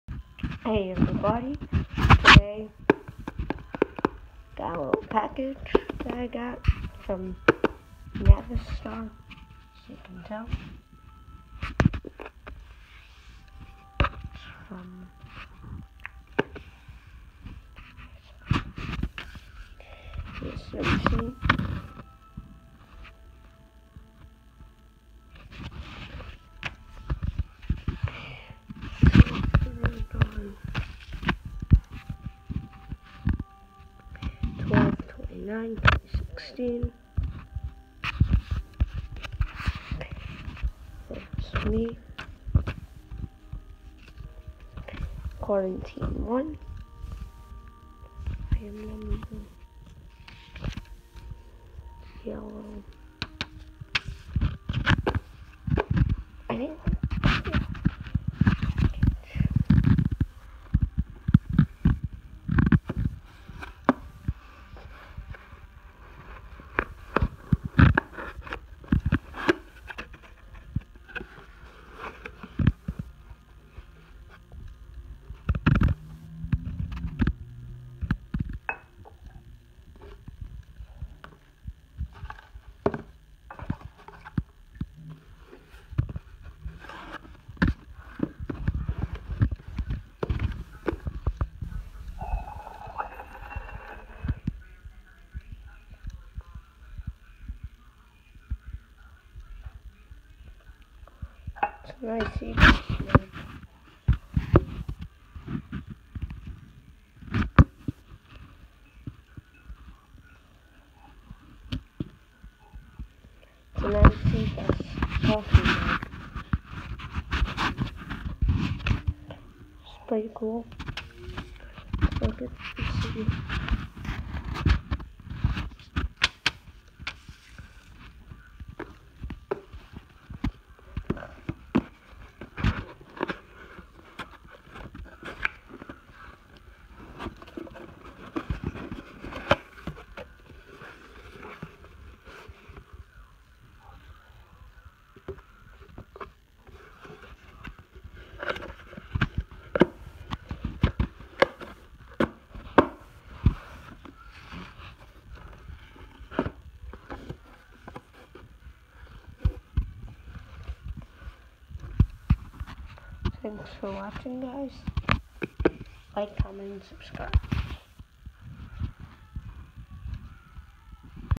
Unboxing a IC Bus coffee mug
Hey guys today we are going to unbox a IC Bus coffee mug